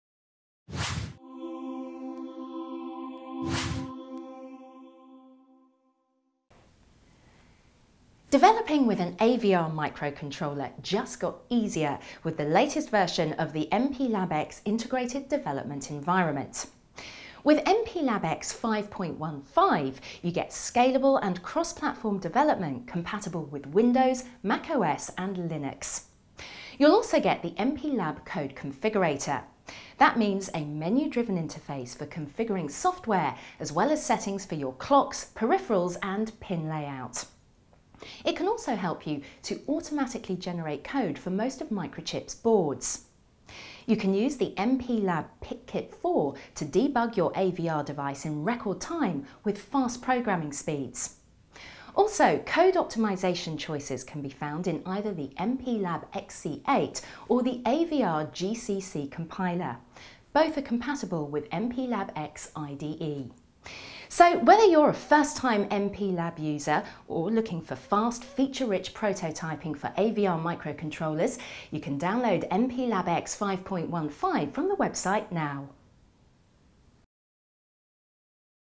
Simplify AVR® development with MPLAB® X 5.15
[MNV353] AVR® Microcontrollers now supported in MPLAB® X Integrated Development Environment